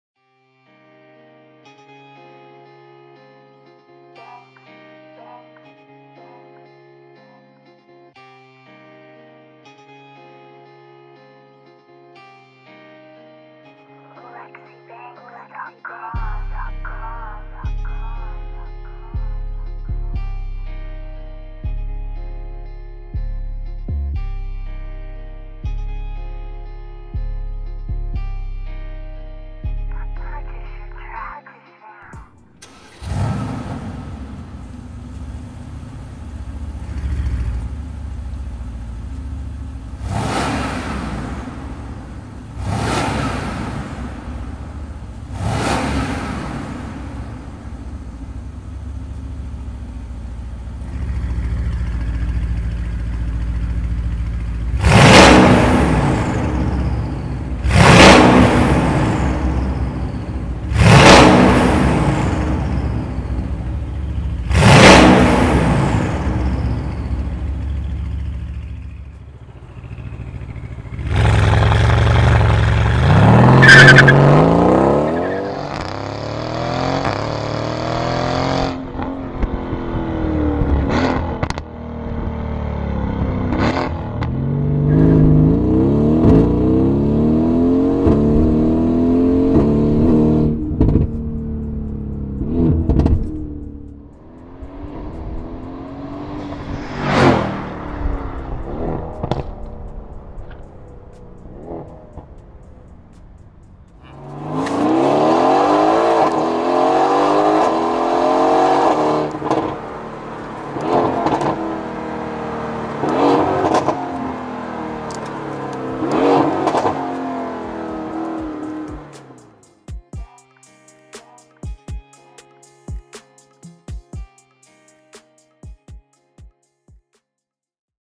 Mercedes-Benz C190 AMG GTS x Fi Exhaust - Cold Start Revs & Acceleration.
Video Fitted with Fi Complete Exhaust System includes:
Ultra High Flow Catless DownPipe